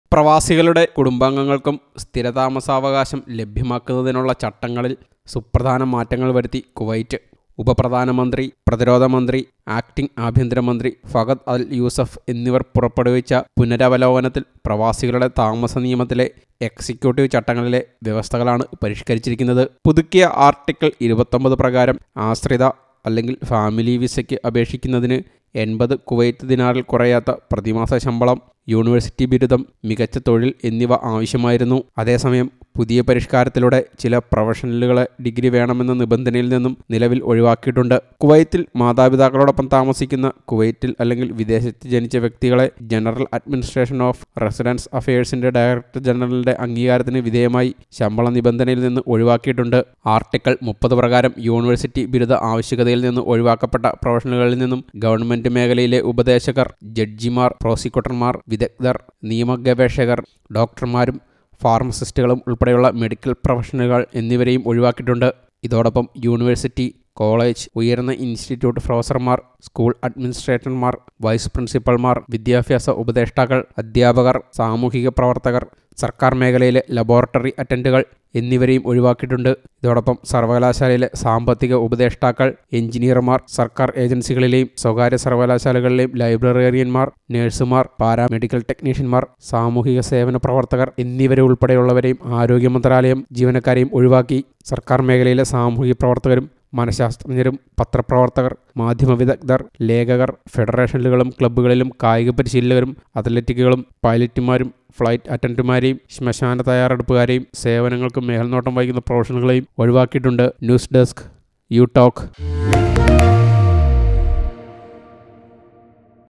വിസാ നിയമങ്ങളില്‍ മാറ്റം വരുത്തി കുവൈറ്റ്
പ്രവാസികളുടെ കുടുംബാംഗങ്ങള്‍ക്കു സ്ഥിരതാമസാവകാശം ലഭ്യമാക്കുന്നതിനുള്ള ചട്ടങ്ങളില്‍ സുപ്രധാന മാറ്റങ്ങള്‍ വരുത്തി കുവൈത്ത്. ഉപപ്രധാനമന്ത്രി, പ്രതിരോധ മന്ത്രി, ആക്ടിങ് ആഭ്യന്തര മന്ത്രി ഫഹദ് അല്‍ യൂസഫ് എന്നിവര്‍ പുറപ്പെടുവിച്ച പുനരവലോകനത്തില്‍, പ്രവാസികളുടെ താമസ നിയമത്തിലെ എക്‌സിക്യൂട്ടീവ് ചട്ടങ്ങള്‍ക്കുള്ളിലെ വ്യവസ്ഥകളാണ് പരിഷ്‌ക്കരിച്ചിരിക്കുന്ന.